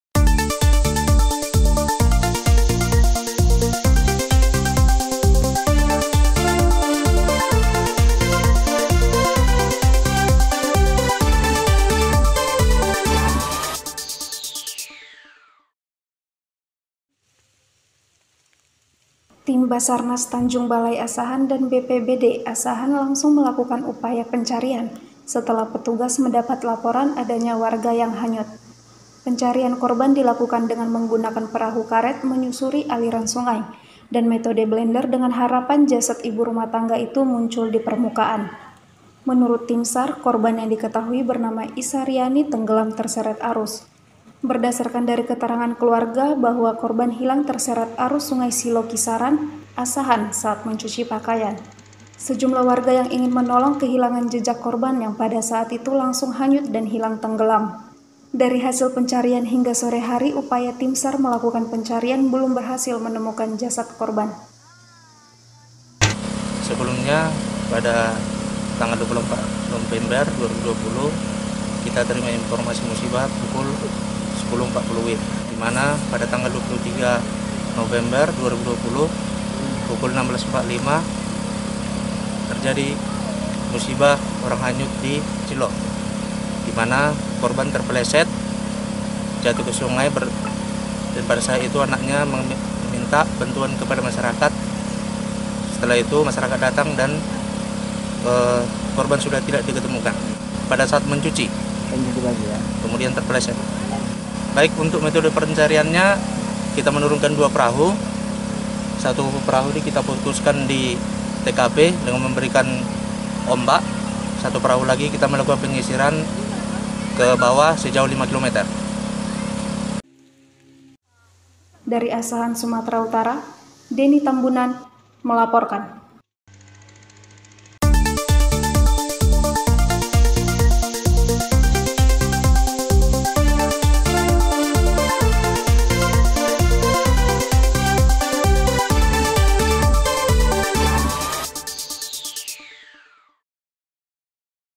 MENCUCI PAKAIAN IBU RUMAH TANGGA HILANG DI TELAN SUNGAI SEI SILAU
Tim basarnas Tanjung Balai Asahan dan BPBD Asahan langsung melakukan upaya pencarian setelah petugas mendapat laporan adanya warga yang hayut.

Pencarian korban di lakukan dengan menggunakan perahu karet menyusuri aliran sungai dan metode blender dengan harapan jasat ibu rumah tangga itu muncul di permukaan.

Menurut timsar korban yang diketahui bernama Ishariyani tenggelam terseret arus.

Berdasarkan dari keterangan keluarga bahwa korban hilang terseret arus sungai Silau Kisaran, Asahan saat mencuci pakaian.

Sejumlah warga yang ingin menolong kehilangan jejak korban yang pada saat itu langsung hayut dan hilang tenggelam.

Dari hasil pencarian hingga sore hari upaya timsar melakukan pencarian belum berhasil menemukan jasad korban.

Tim Liputan : Deni Tambunan

Salam Tribrata.